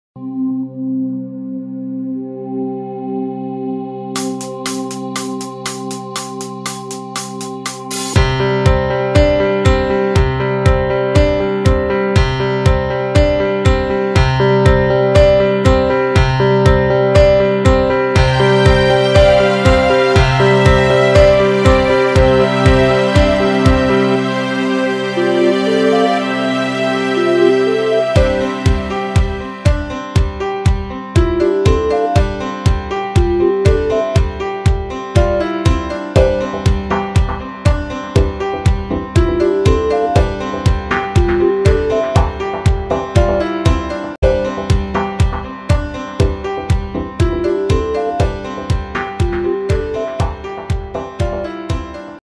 The abandoned shoes
This is a video I made as a school project. I made the music and the video myself, enjoy.